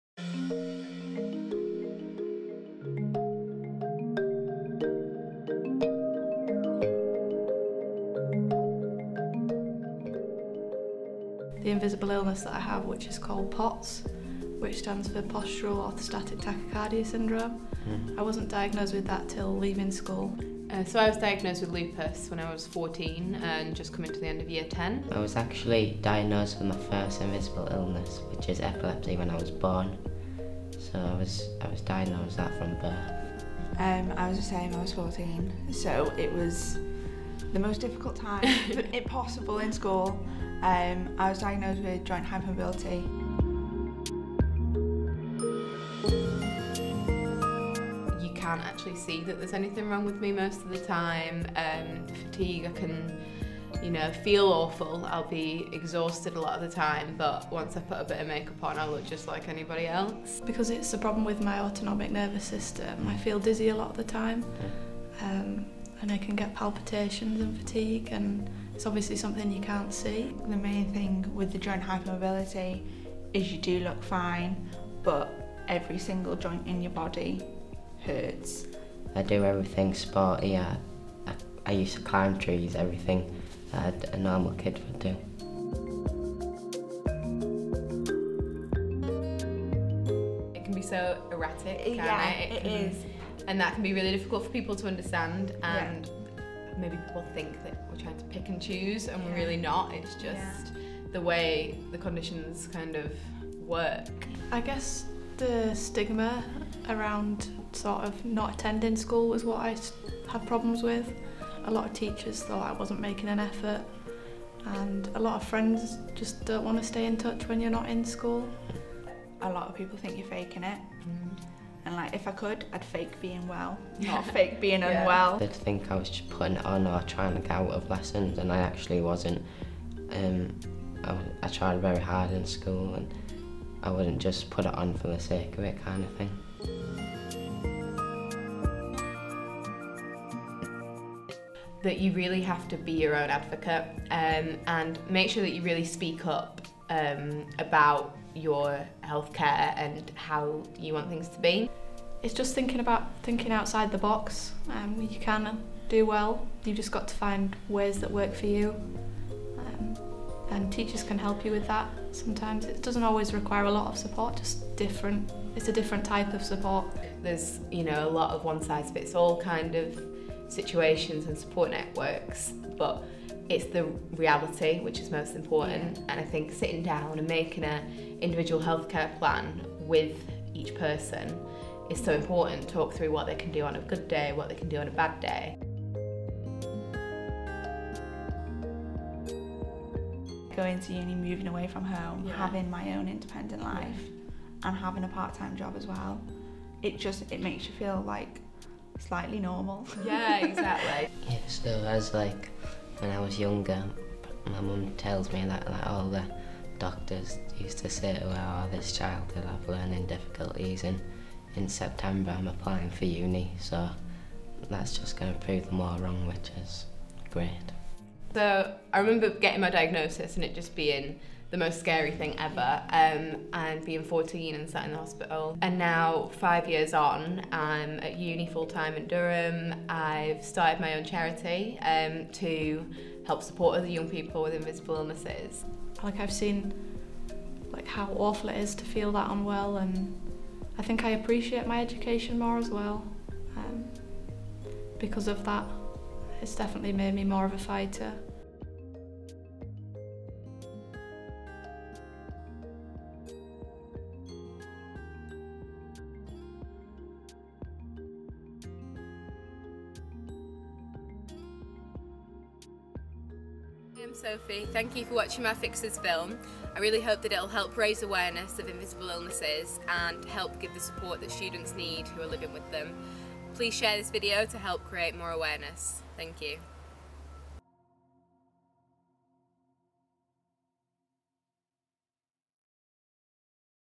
See My Invisible Disability
Sophie Ainsworth, 19, wants to show young people how it feels to have an illness people can't see or don't understand.